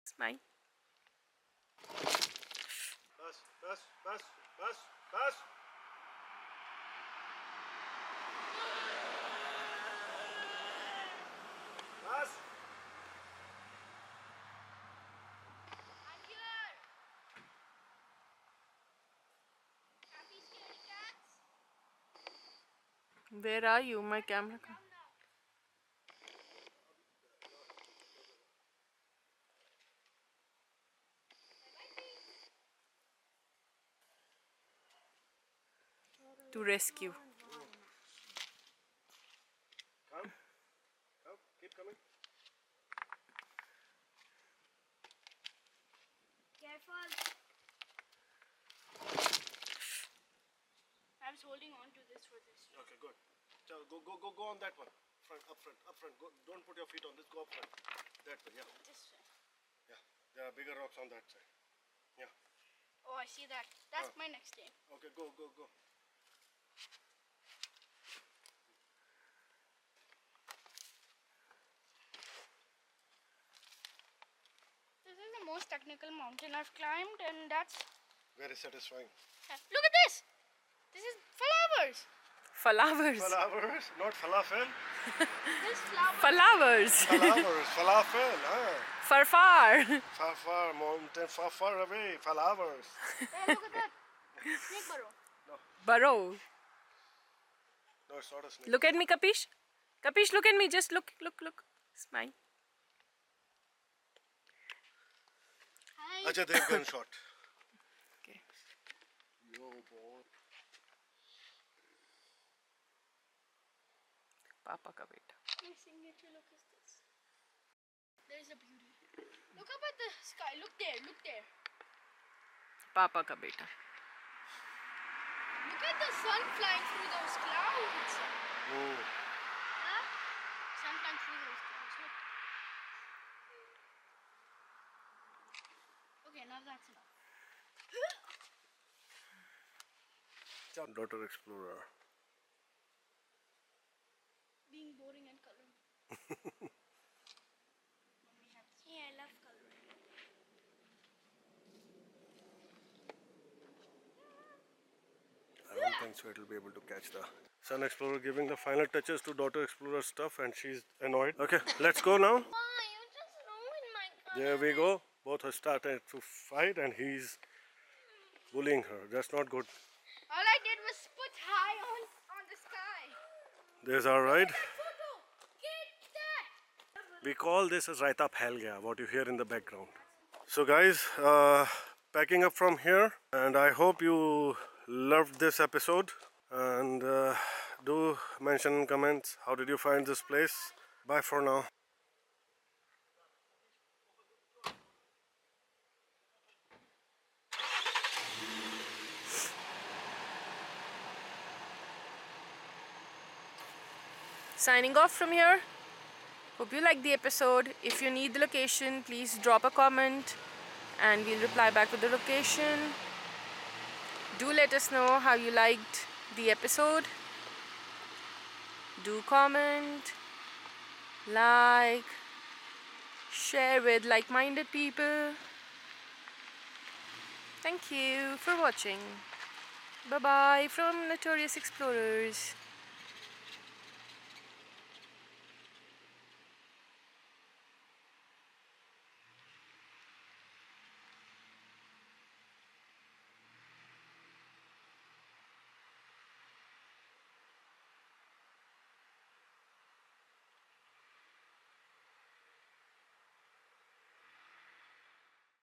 Son Explorer almost slipped while descending in wadi far far Rescue by father explorer | weird smile
Brining an end to our 3 episode series of wadi farfar exploration. there are good trekking opportunities available in the wadi.